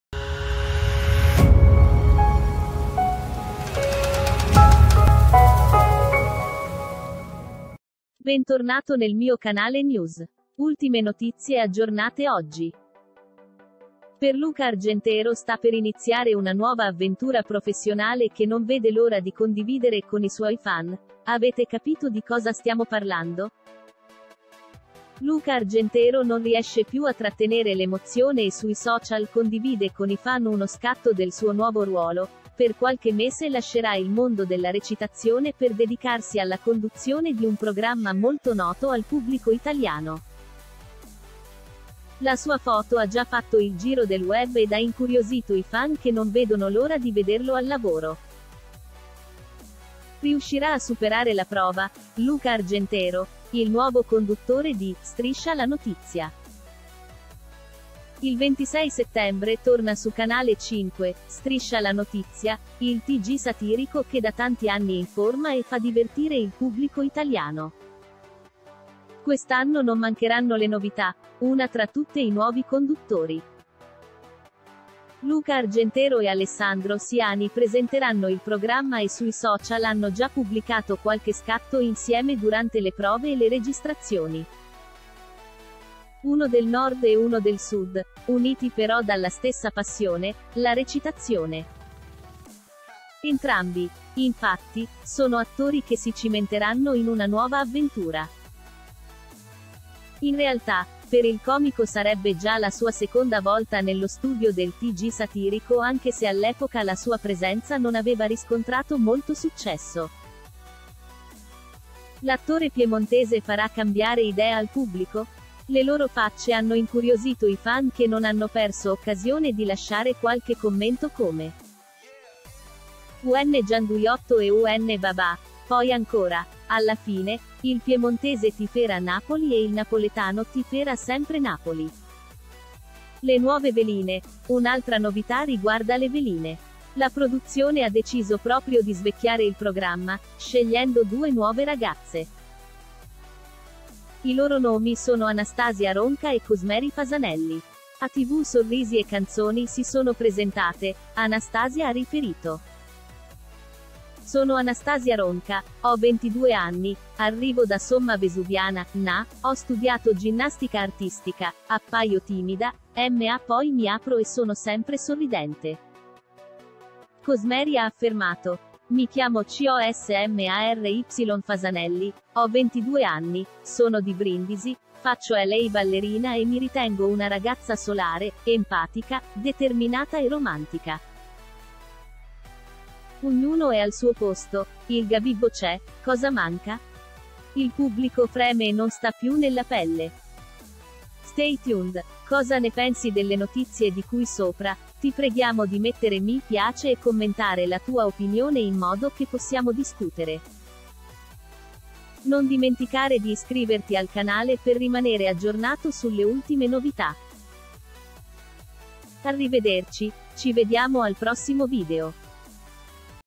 Luca Argentero esce allo scoperto, cambia lavoro cosa farà adesso
Luca Argentero non riesce più a trattenere l’emozione e sui social condivide con i fan uno scatto del suo nuovo ruolo: per qualche mese lascerà il mondo della recitazione per dedicarsi alla conduzione di un programma molto noto al pubblico italiano.

La sua foto ha già fatto il giro del web ed ha incuriosito i fan che non vedono l’ora di vederlo al lavoro. Riuscirà a superare la prova?

Luca Argentero, il nuovo conduttore di “Striscia la notizia”
Il 26 settembre torna su Canale 5 “Striscia la notizia”, il tg satirico che da tanti anni informa e fa divertire il pubblico italiano. Quest’anno non mancheranno le novità, una tra tutte i nuovi conduttori.